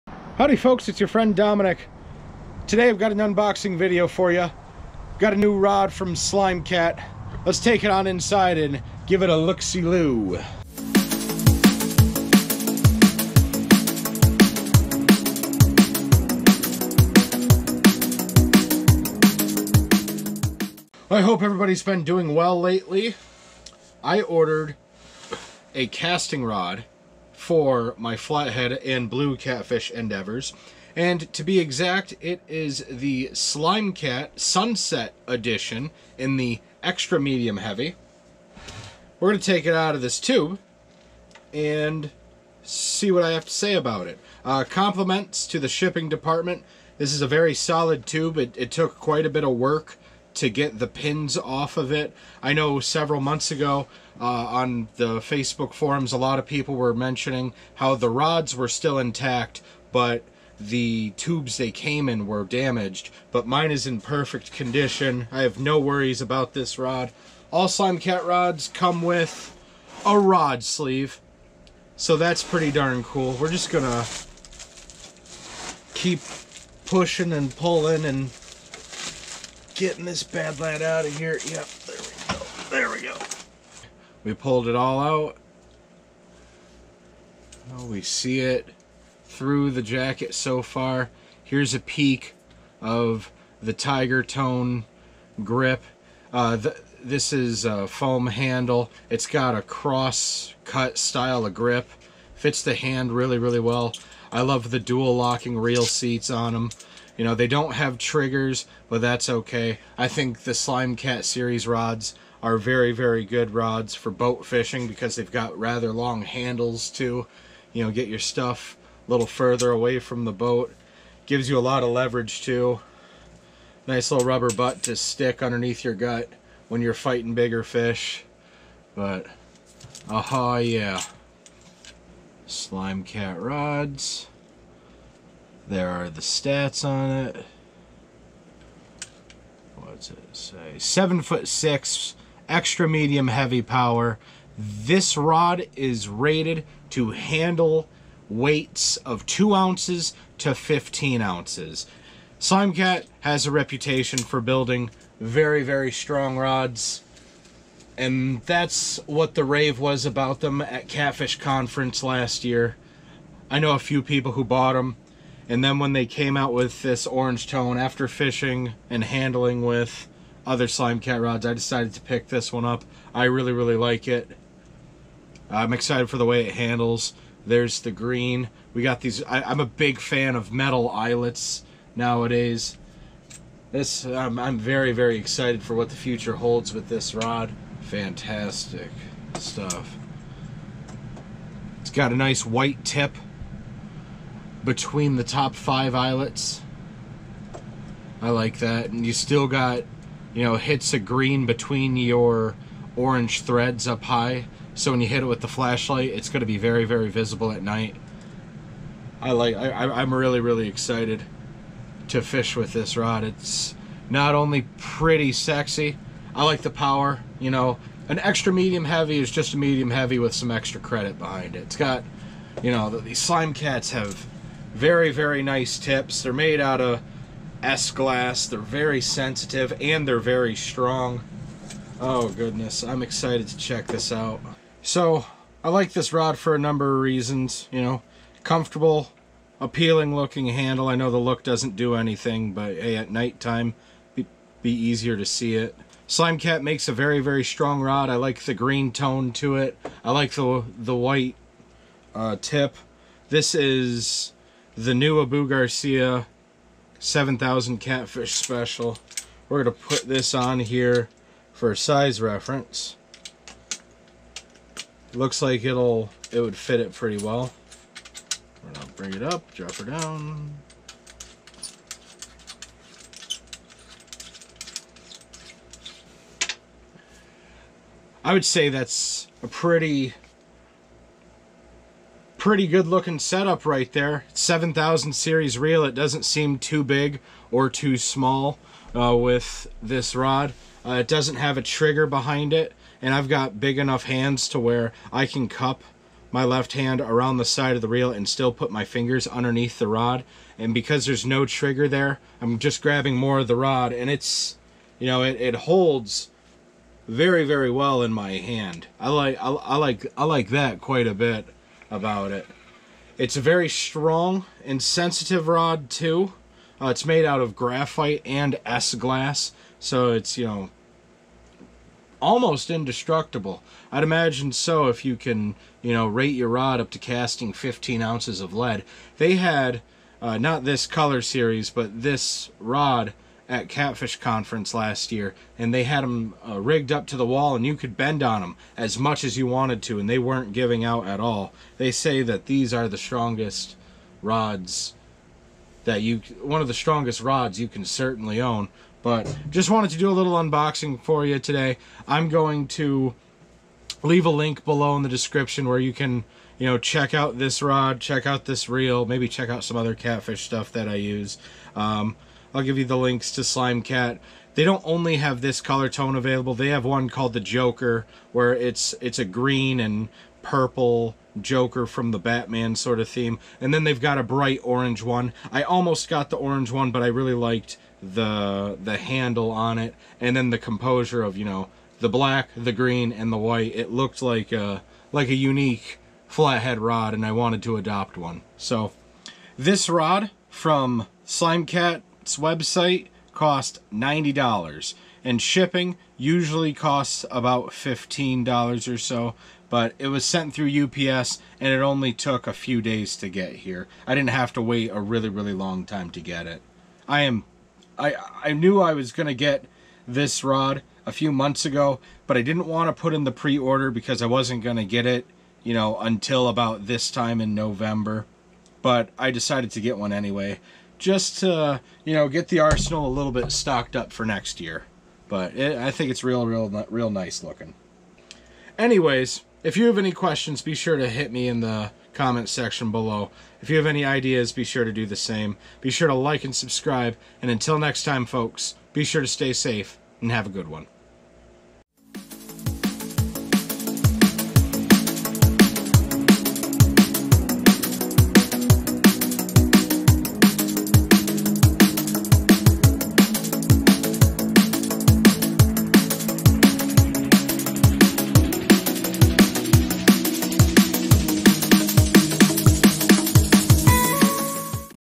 Slimecat Sunset Rod Unboxing- Special Edition Catfish Rod for 2021
I’m very excited to use this rod over the next season of chasing whisker critters. Slimecat makes a very nice rod for all catfish purposes. They’re incredibly sensitive to indicate bites and has a super solid build to withstand the strength of a real river monster.